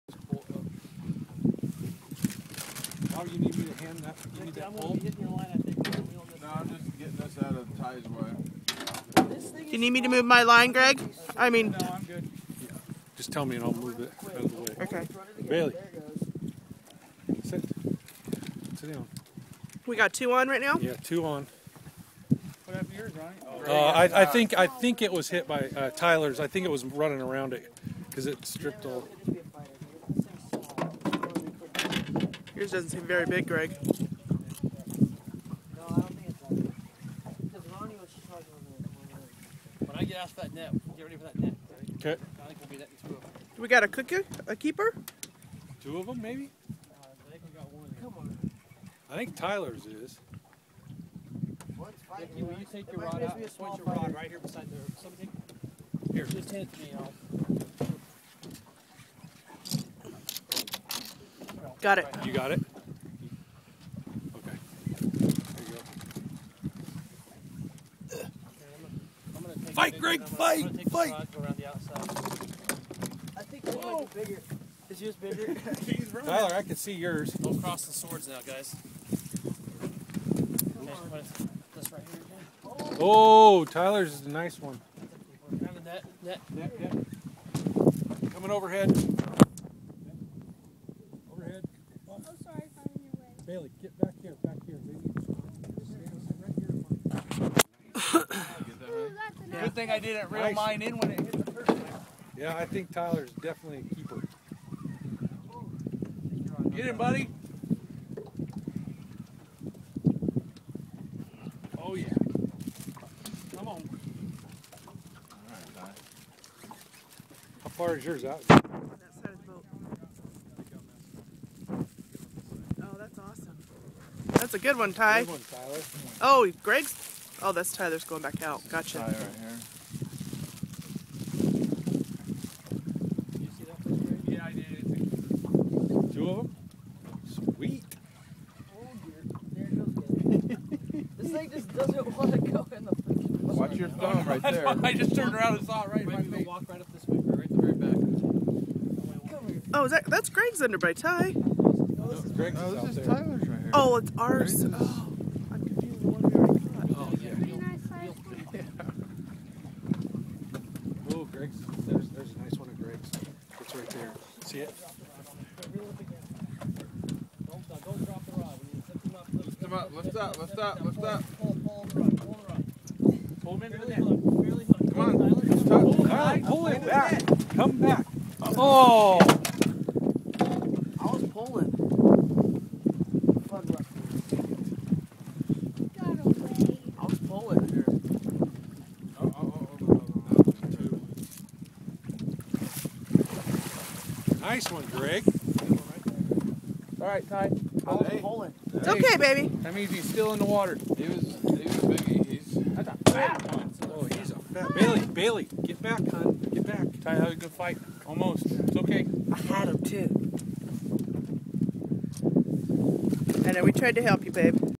Sturgeon Fishing With Troy Corgain of Angler's Choice Guide Service.
Greg and Tyler Mellor reeling their fish at the same time. Tyler didn't land his catch but I guess you can't keep them all. Great job Ty for getting him that far, he was a biggy. What a great day.